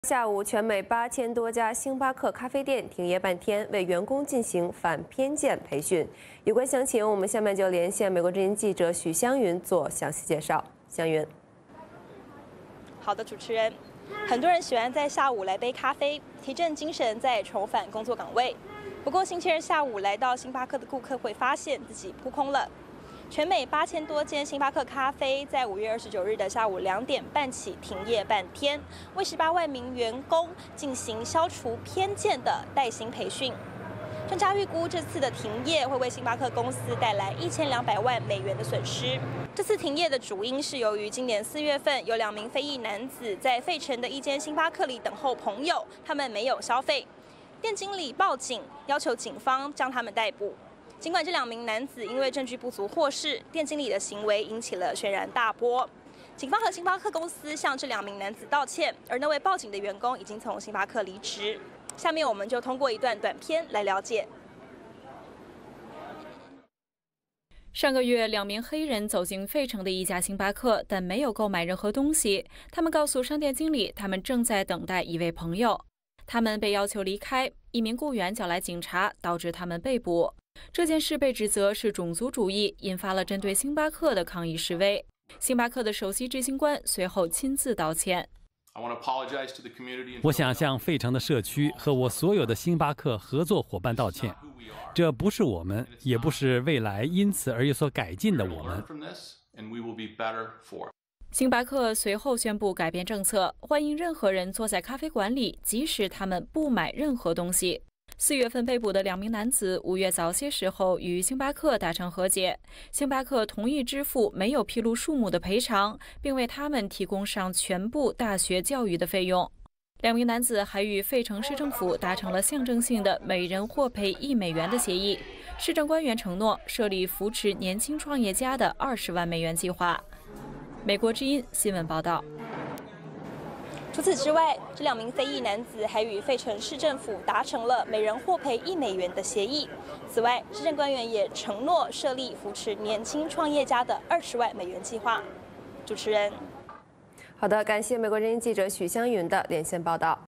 VOA连线(许湘筠):星巴克八千分店停业半天 进行反偏见教育
为了平息抗议的呼声，美国连锁咖啡店星巴克(Starbucks)的8000家美国分店星期二下午暂时停业，对员工进行反偏见培训。上个月，星巴克一家分店的雇员报警，导致店内的两名黑人被捕，引发抗议。